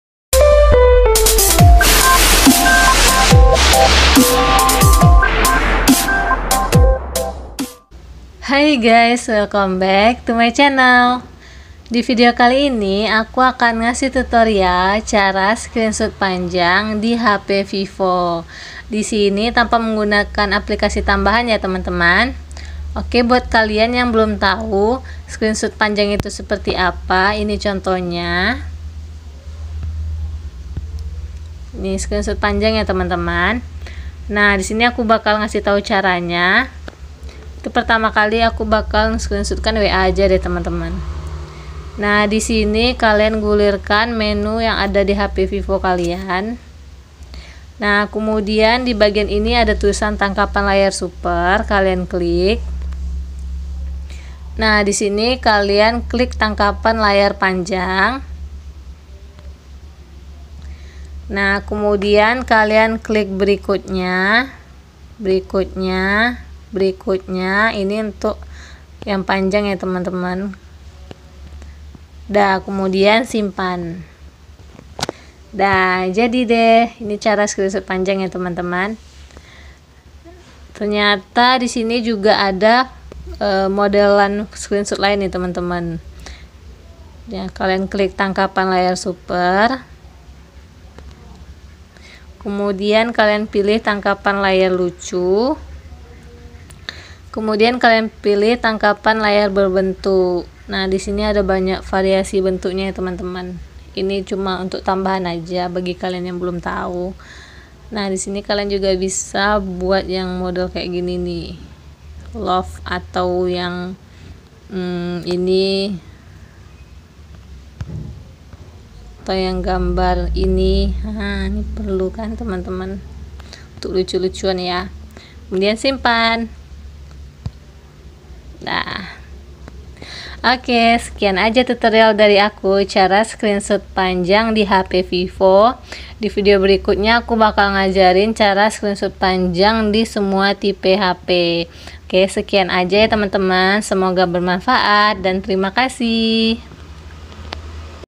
Cara Screenshot Panjang Di Hp Vivo
Hai Gaes seperti janji aku di video aku sebelumnya, di video aku kali aku akan ngasih tutorial cara screenshot panjang di hp vivo. Tutorial screenshot panjang ini tanpa menggunakan aplikasi tambahan ya gaes.